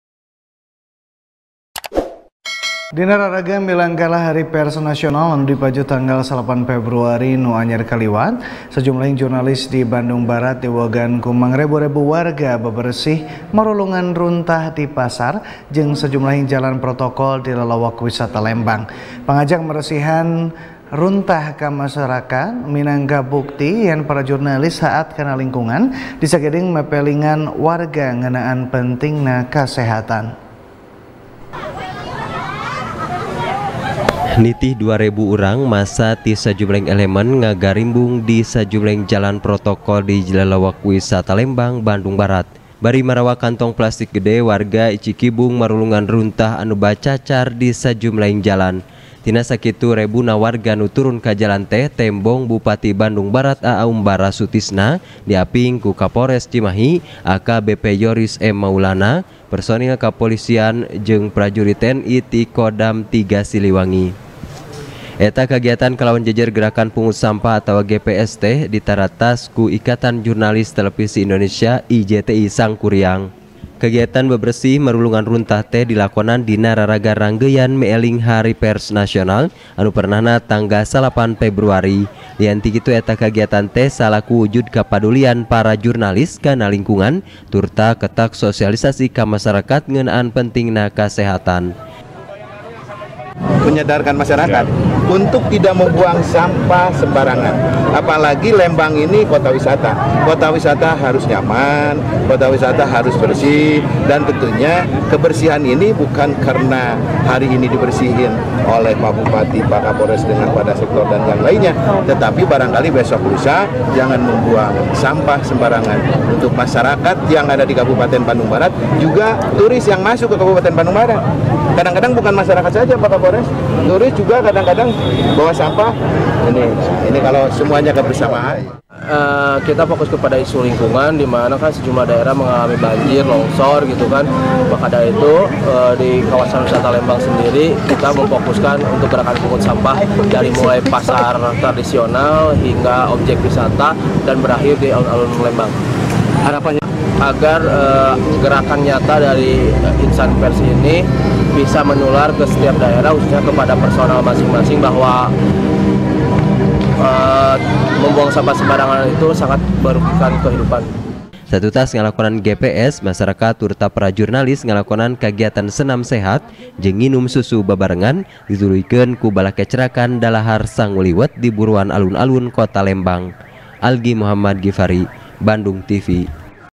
Mieling Hari Pers Nasional 2020, Marulungan Runtah Di Lelewek Wisata Lembang | TP | BANDUNGTV
TP | BANDUNGTV | DERI FIRMAN | Dina Raraga Milangkala Hari Pers Nasional Anu Dipaju Tanggal Salapan Februari Nu Anyar Kaliwat, Sajumlahing Jurnalis Di  Bandung Barat, Diwewegan Ku MangrebuRebu Warga  Beberesih Marulungan Runtah Di Pasar Jeung Sajumlahing Jalan Protokol Di Lelewek Wisata Lembang. Pangajak Meresihkeun Runtah Ka Masarakat, Minangka Bukti Yen Para Jurnalis Haat Kana Lingkungan, Di Sagedengeun Mepelingan Warga Ngeunaan Pentingna Kasehatan.